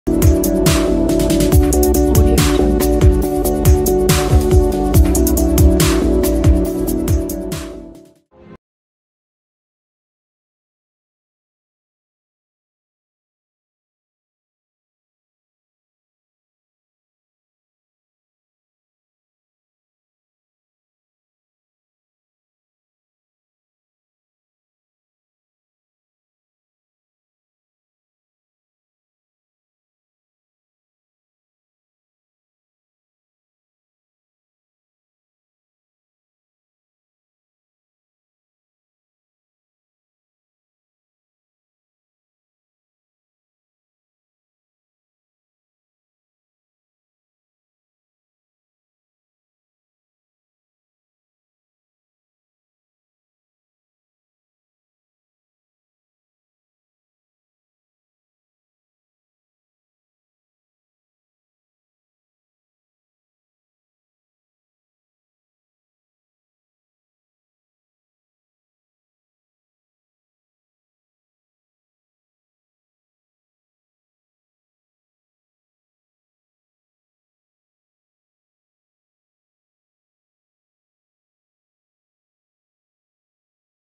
voice of kutch mundra audition promo
Blessings Events presents "Voice of kutch" singing sensation 2019.
Mundra Audition 2019 Date:17Aug 2019